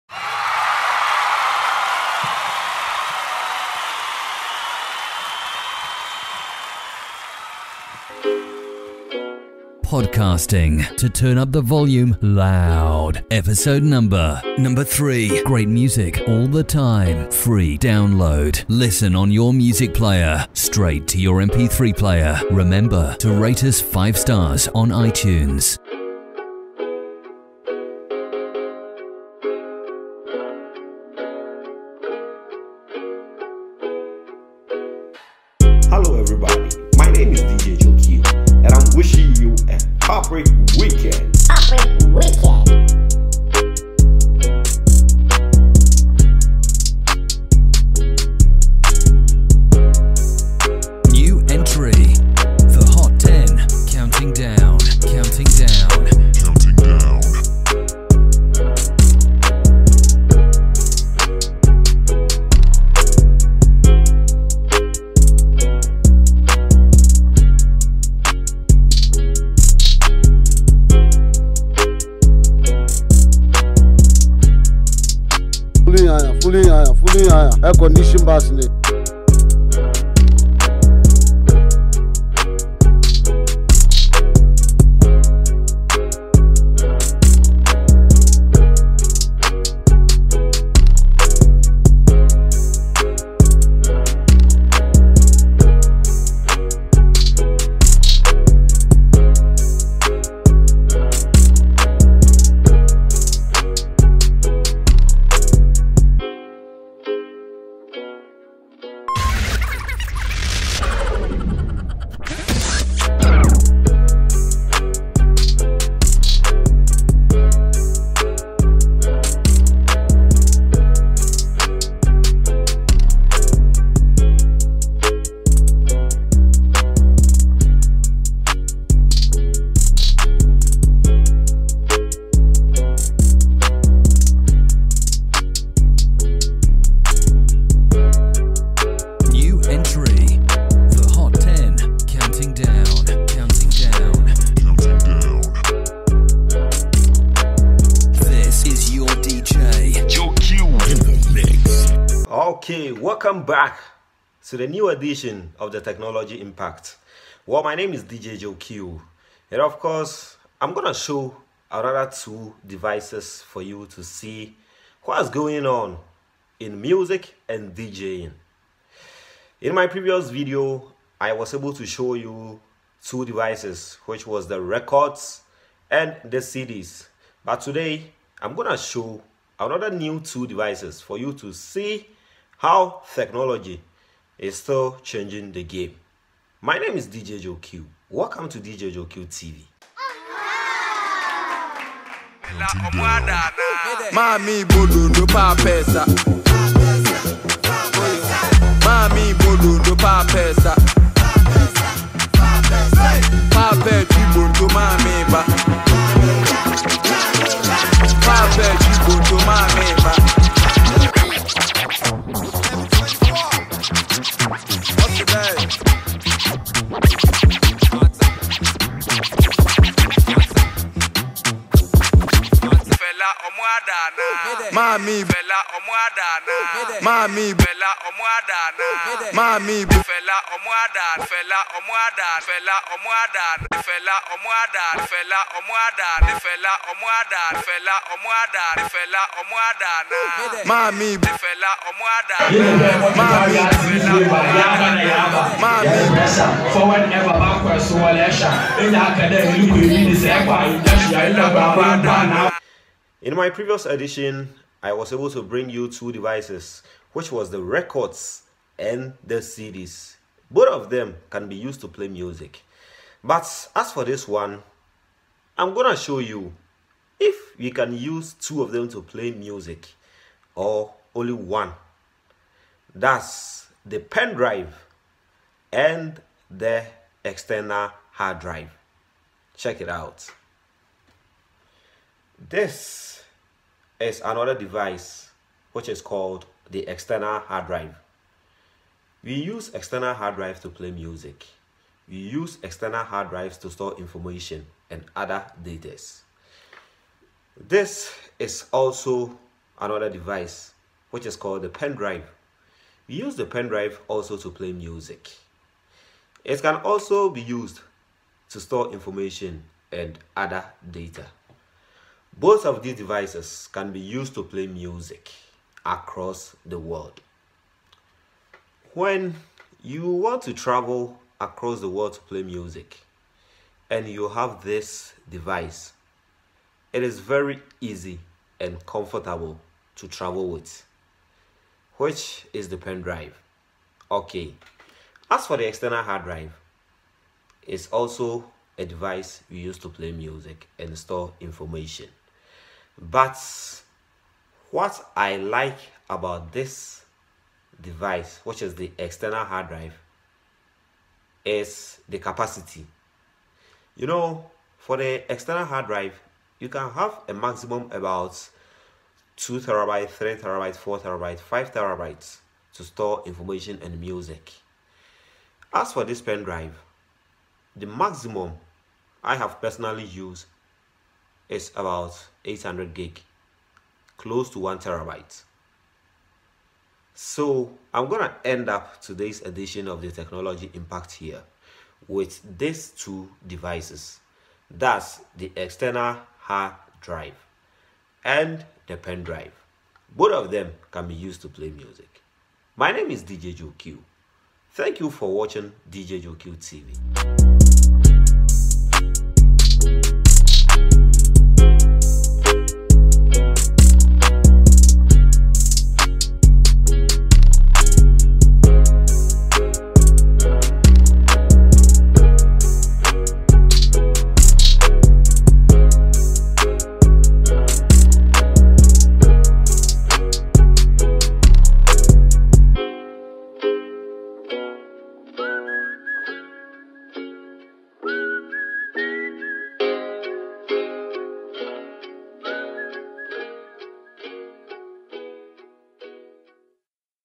How to know if External Hard drives can play music and shows.
Pen drives and External Hard drives can easily be used to play music at all events. share your comments with me on this Episode